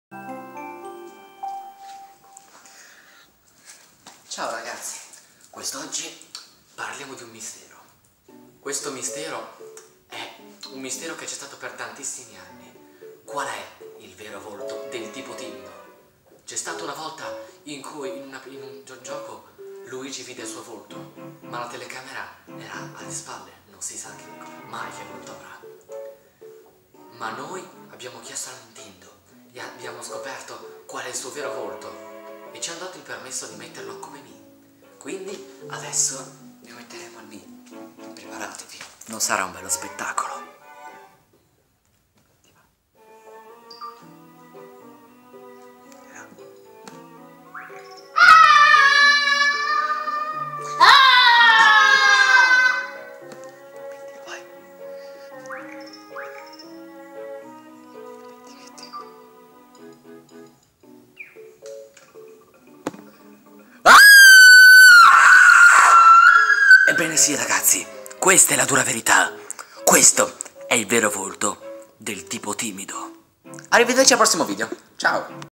Il vero volto del TipoTimido
E con una nuova telecamera vi faremo scoprire uno dei più grandi misteri della storia di Nintendo: il vero volto del Tipo Timido!
E come direbbe Mariello Prapapappo, MISTERO RISOLTO!
BRAVO MATTIELLO!